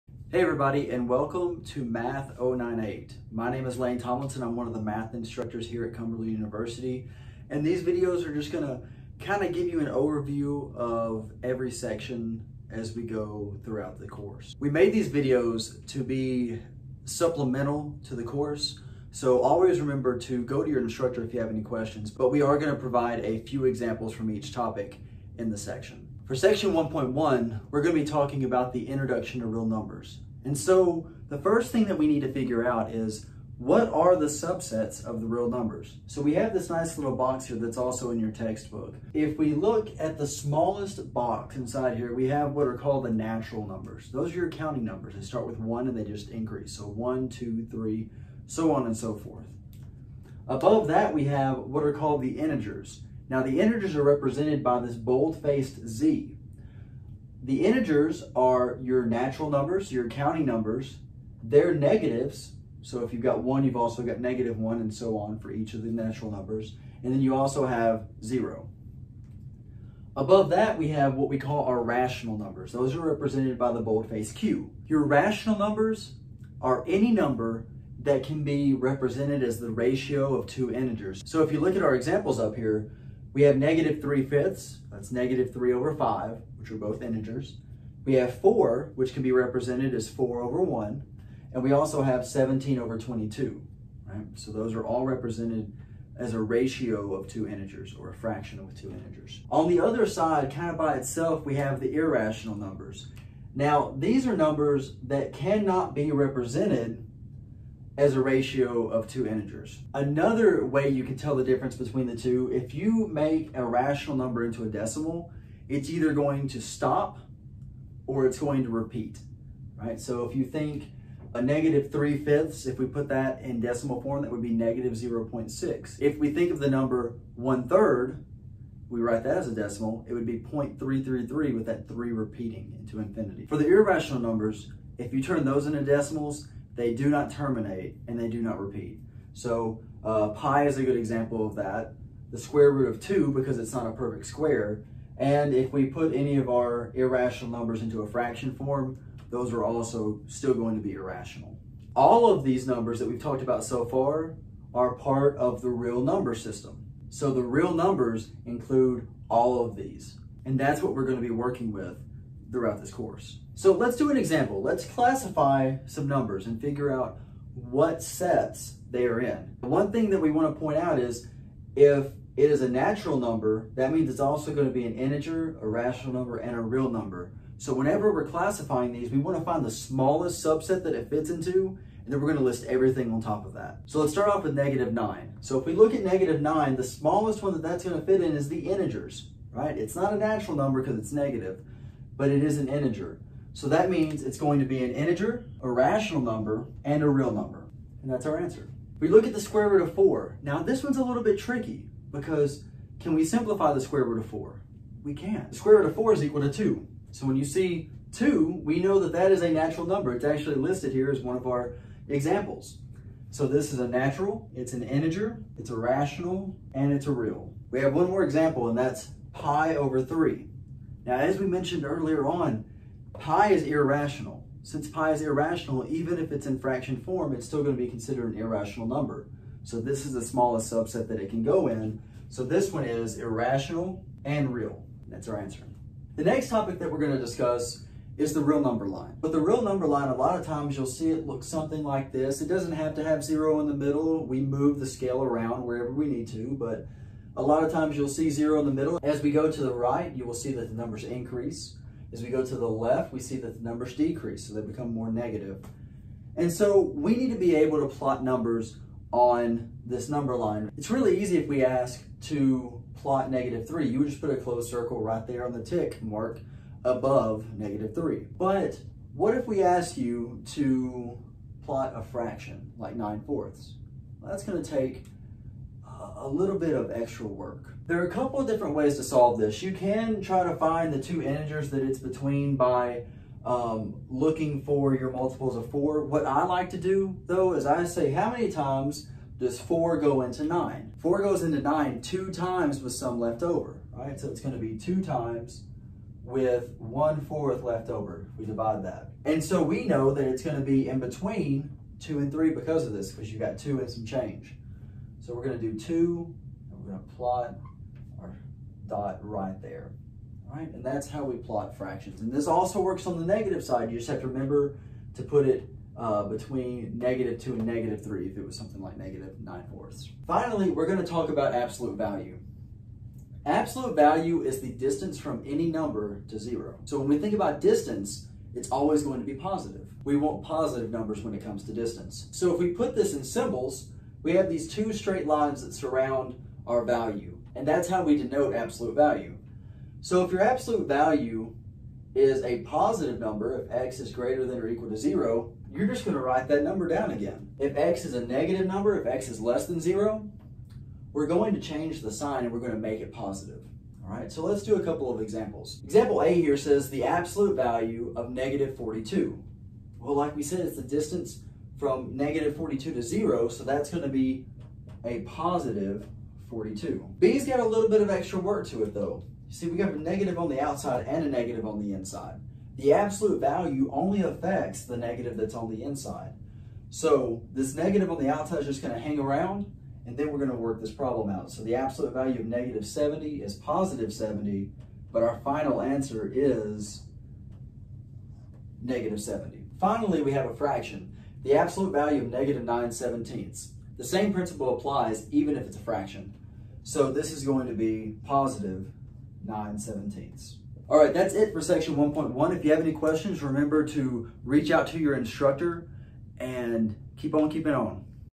Section 1.1: The Real Number System
Section 1.1 covers the subsets of real numbers, the real number line, and absolute value.

Sections:
0:00 Introduction
0:27 Subsets of Real Numbers
2:43 Classifying Real Numbers
3:55 The Real Number Line
4:29 Plotting Fractions on the Number Line
5:20 Absolute Value